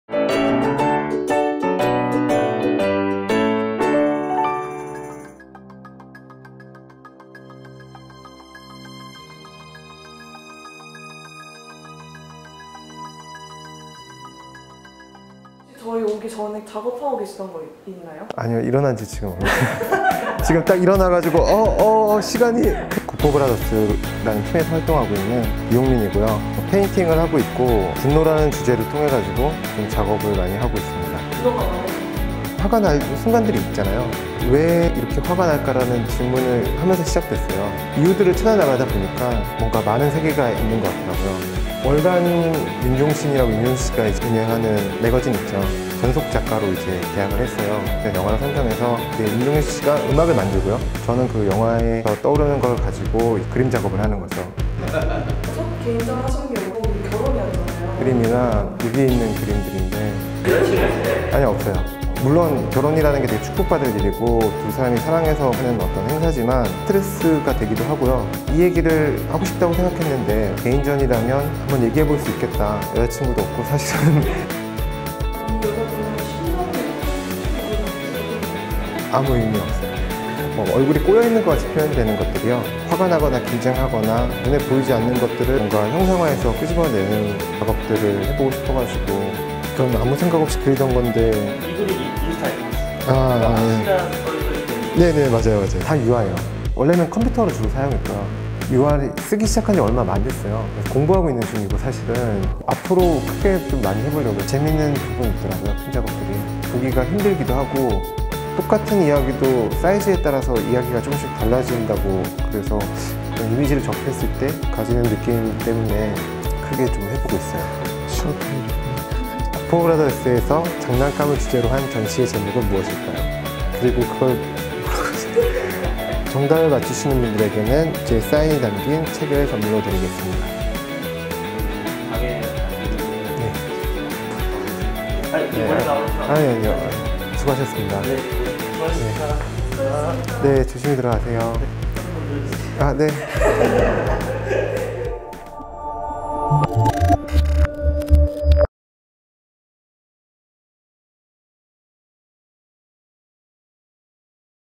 [ENG_아티스트 이홍민] 그림이 화를 낸다 부들부들
사소한 문제부터 커다란 걱정거리까지, 우리를 분노하게 하는 것들이 많이 있죠? 그런 분노를 흰 종이에 풀어내는 작가 이홍민을 The ICONtv가 만나고 왔는데요. 뒤틀린 표정과 과격한 표현법! 마니아들이 특히 좋아하는 독특한 개성의 아티스트 이홍민의 이야기를 ‘아웃 레이지’ 티비에서 만나보세요!